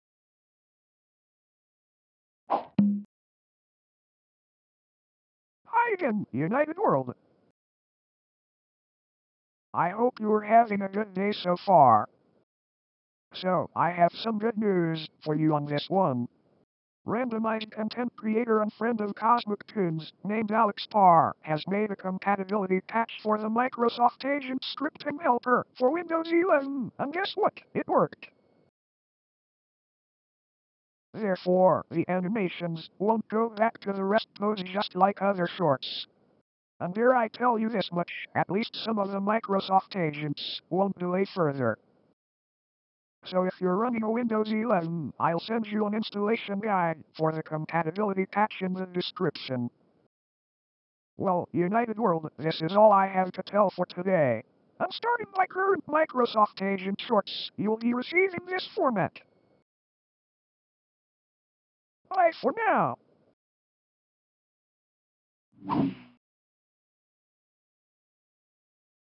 A message to United World.
I applied the same Windows 10 treatment on my Windows 11.
For @UnitedWorld135.